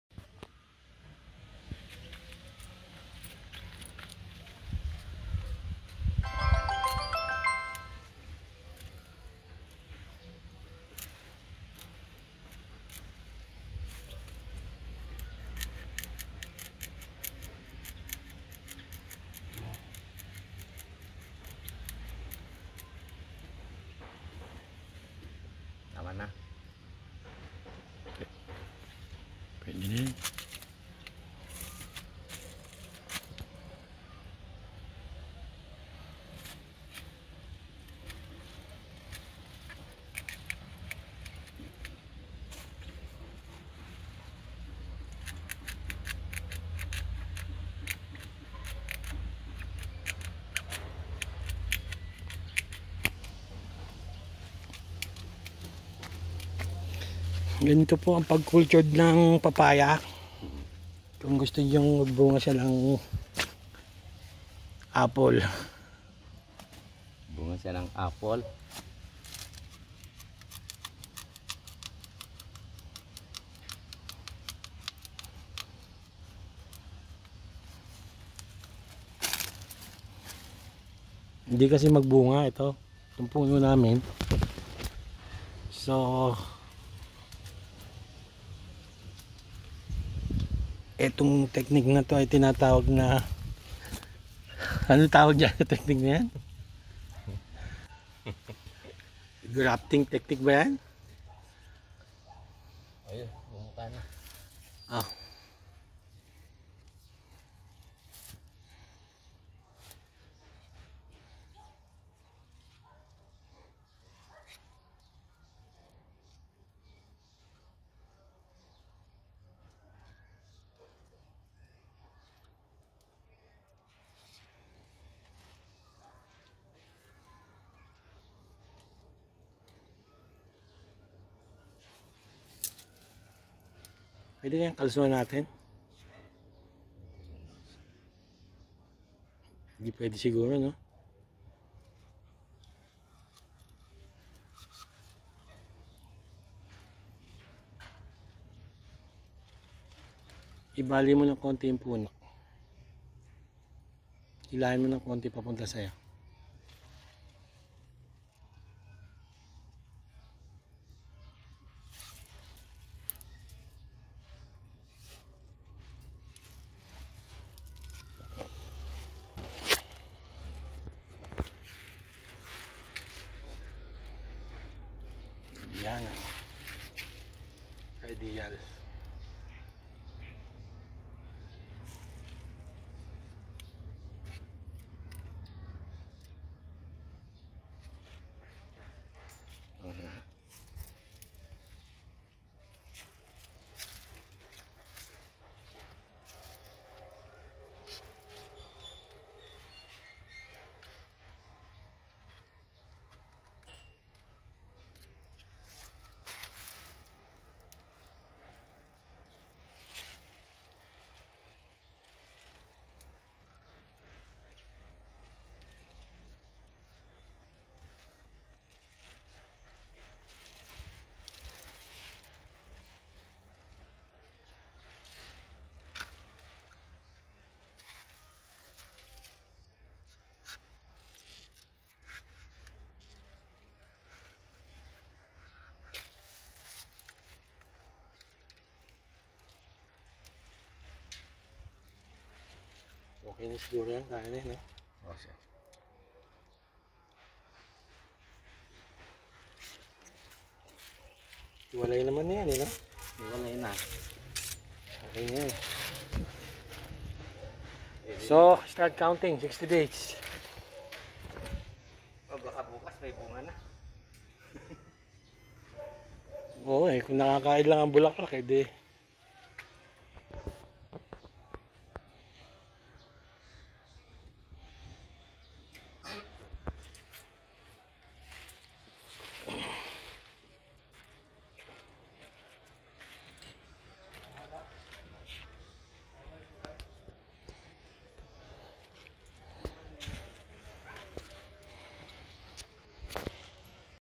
papaya tree grafting
Thanks to Rene Berbano for torturing the very peaceful tree.
60 days waiting period if the tree will survive the ordeal.